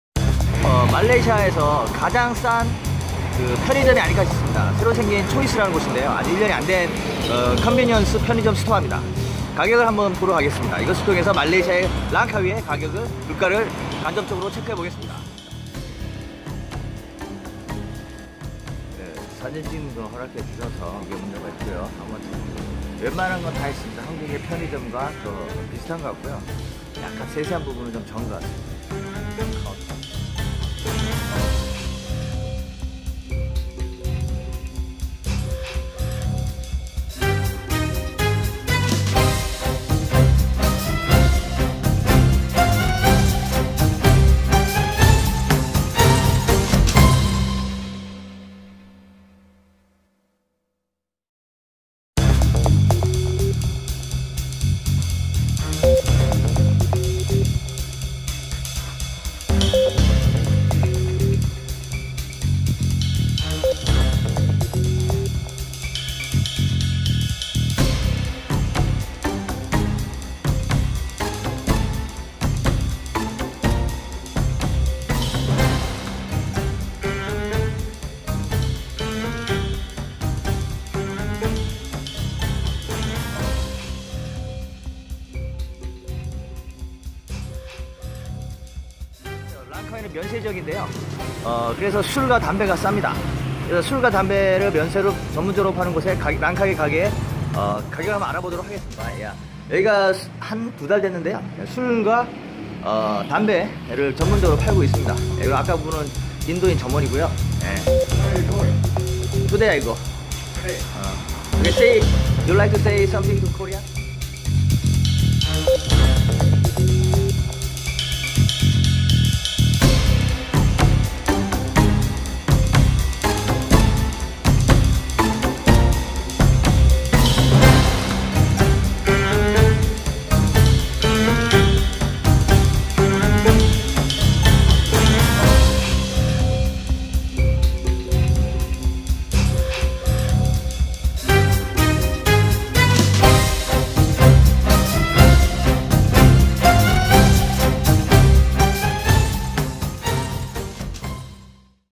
(말레이시아 여행 물가편, 랑카위 물가 체크. Checking the langkawi price in convenience store
말레이시아 물가편에서 이번에는  랑카위 물가를(담배, 콜라, 술) 체크해 봤습니다. 전 세계적으로 기준이 될 만한 아이템 들의 가격을 말레이시아 랑카위 편의점에서 확인해 보았습니다. checking  malaysia langkawi price (alchol, tabaco, coke etc)